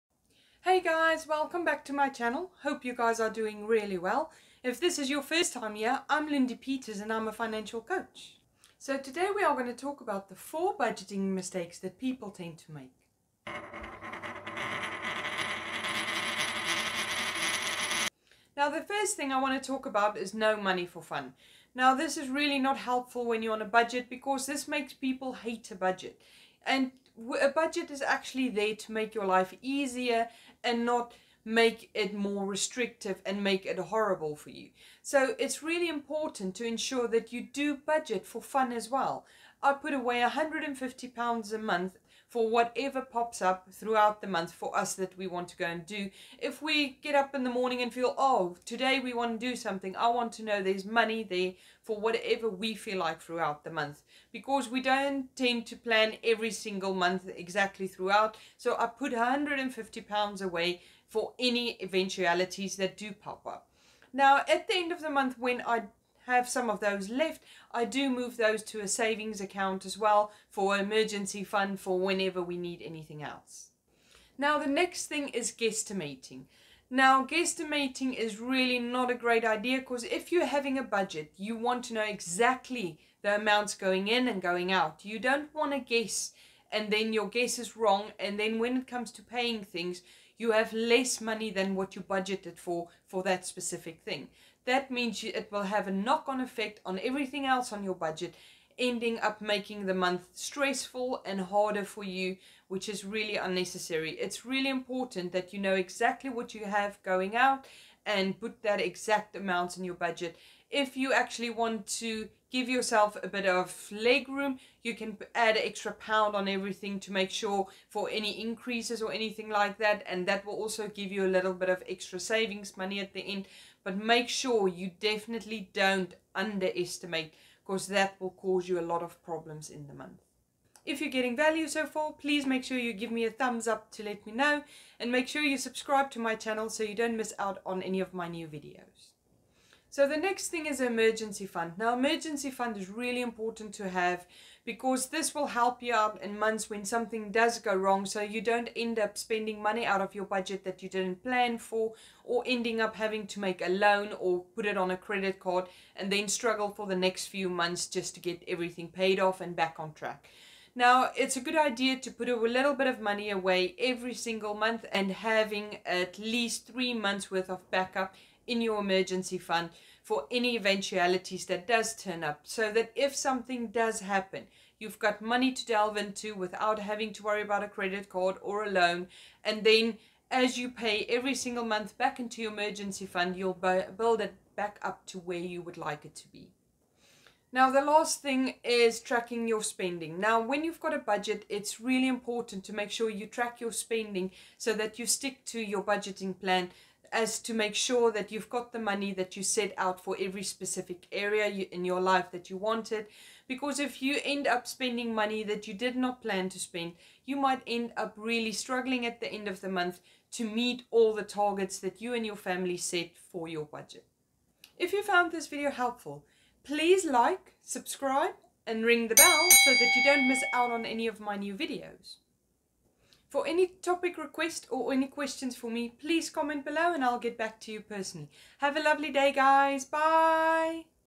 Top 4 Budgeting Mistakes that gets people down
The Top 4 Budgeting Mistakes people make.

1. No Fun
2. Guesstimating
3. Emergency fund
4. No Tracking

📈 Start investing: 📈
Join Trading 212 (Get a Free Stock Share worth up to £100)